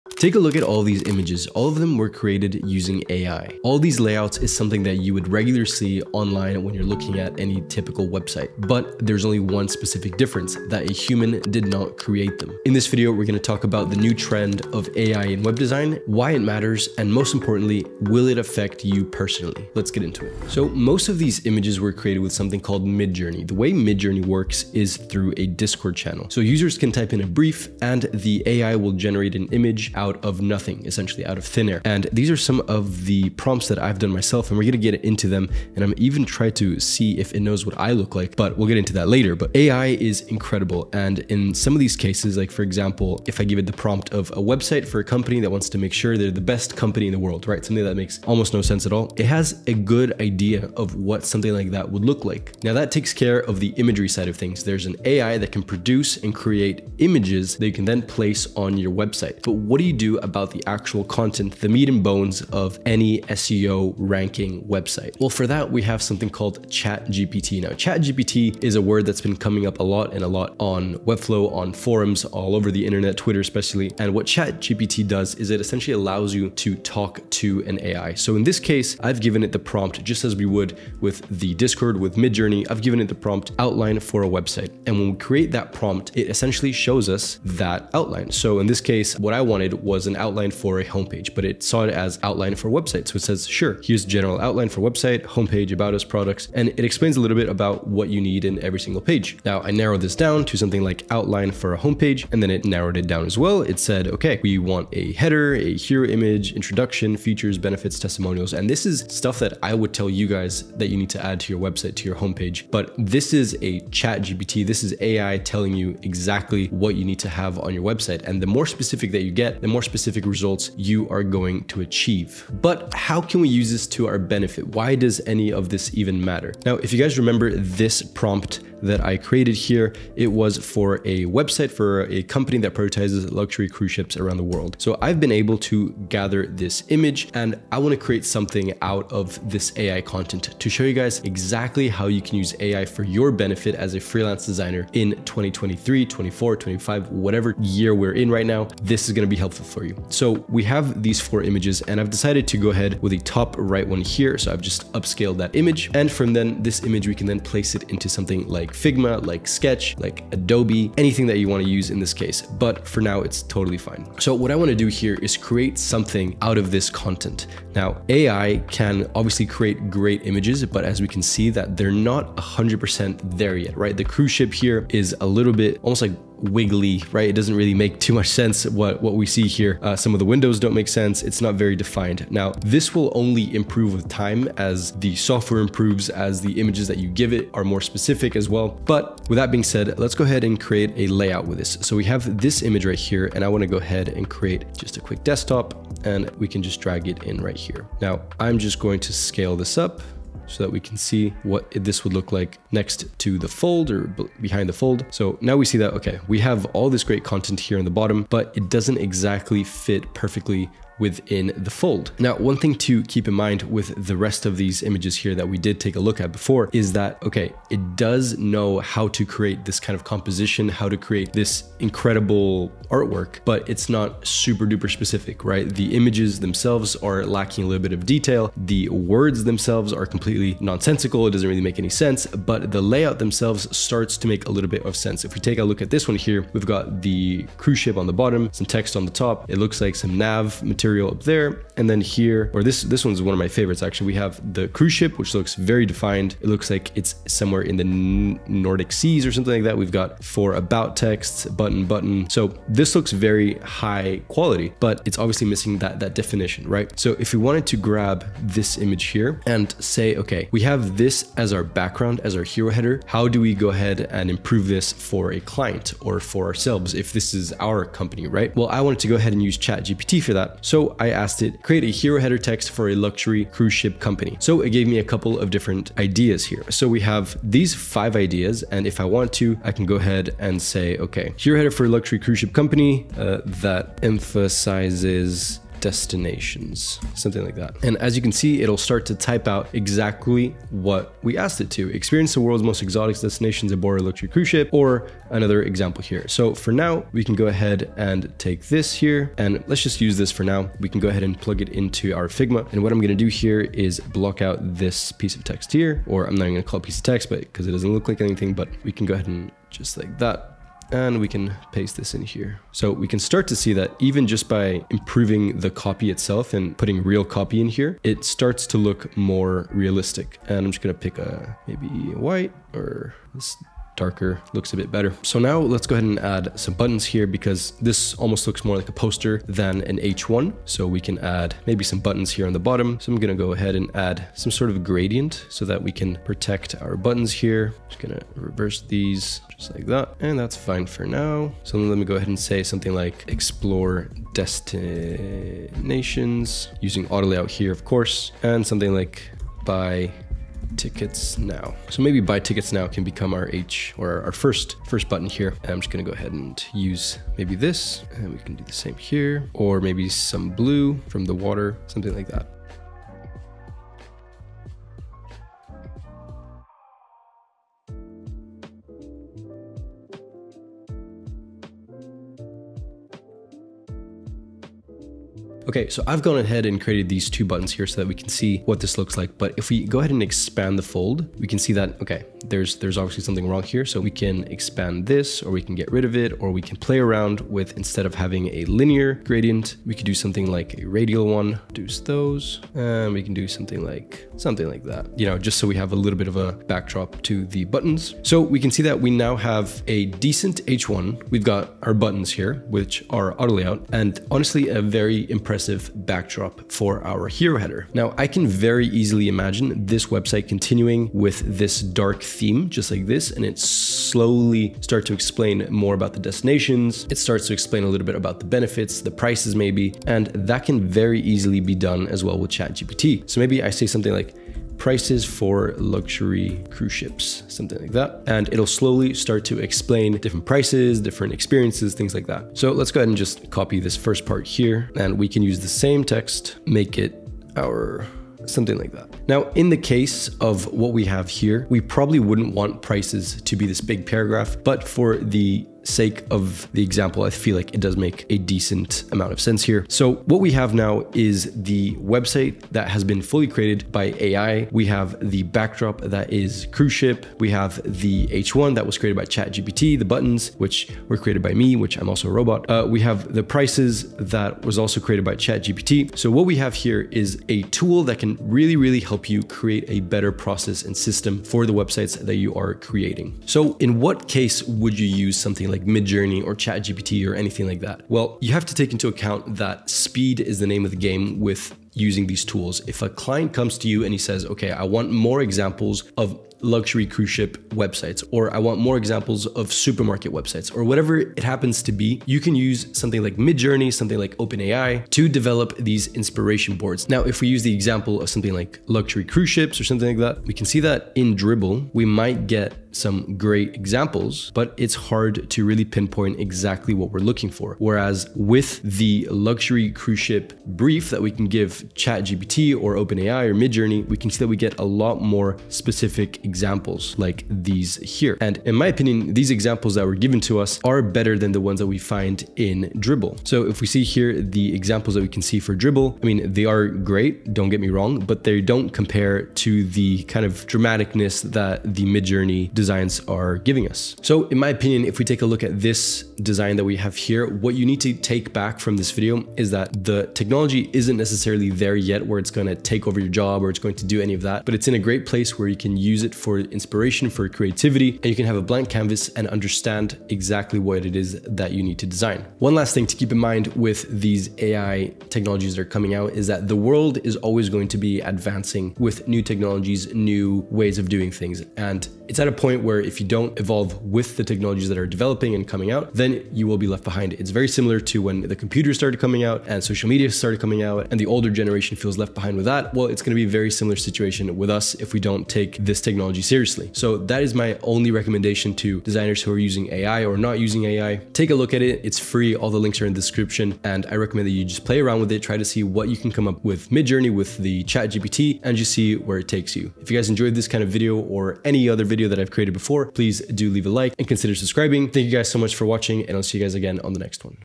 AI Web Design: This changes EVERYTHING (ChatGPT & Midjourney)
Ai in web design will change the way we create layouts and present ideas. Understanding the power of chatgpt and midjourney can help you create great work faster and come up with new ideas more often. In this video, we're taking a look at how we can use AI in our workflow to improve the efficiency of our designs.

0:00 Intro
0:20 Midjourney
1:00 ChatGPT
2:00 Using AI To Create a Layout
8:00 Use Cases For You
9:25 Final Words